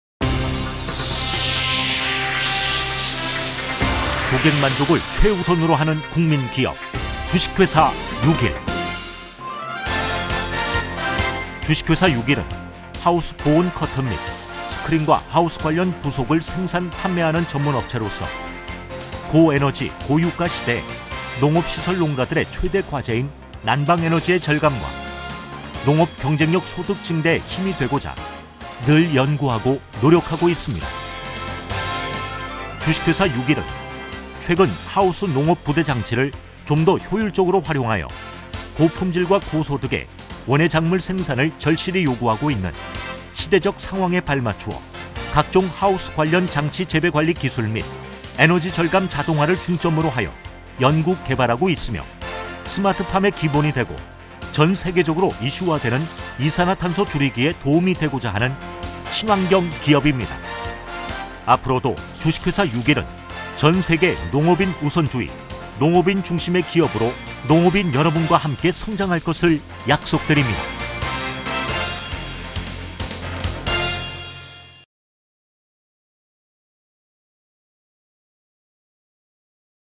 (주)육일 _회사소개_KOR
㈜육일은 하우스 보온 커튼 및 스크린과 하우스 관련 부속을 생산, 판매하는 전문 업체로서 고에너지, 고유가 시대에 농업 시설 농가들의 최대 과제인 난방 에너지의 절감과 농업 경쟁력 소득증대에 힘이 되고자 늘 연구하고 노력하고 있습니다.

㈜육일은, 최근 하우스 농업 부대 장치를, 좀 더 효율적으로 활용하여, 고품질과 고소득의, 원예작물 생산을 절실히 요구하고 있는, 시대적 상황에 발맞추어, 각종 하우스 관련 장치 재배 관리 기술 및 에너지 절감 자동화를 중점으로 하여, 연구 개발하고 있으며, 스마트팜의 기본이 되고, 전 세계적으로 이슈화되는 이산화탄소 줄이기에, 도움이 되고자 하는, 친환경 기업입니다

앞으로도 ㈜육일은 전세계의 농업인 우선주의, 농업인 중심의 기업으로 농업인 여러분과 함께 성장할 것을 약속 드립니다.

고객만족을 최우선으로 하는 국민기업
㈜육일